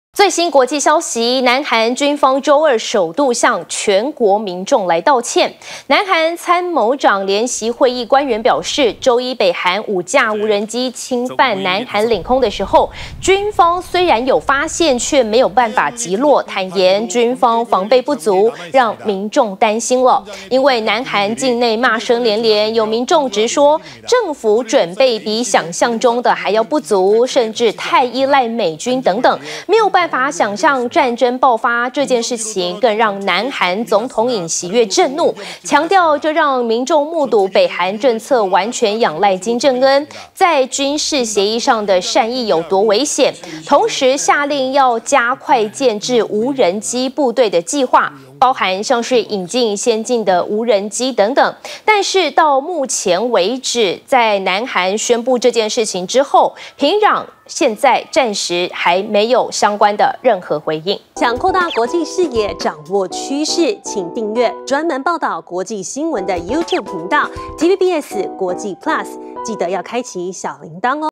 未擊落北韓無人機 南韓軍方挨轟後道歉｜TVBS新聞@internationalNewsplus
南韓軍方周二，首度向全國民眾道歉。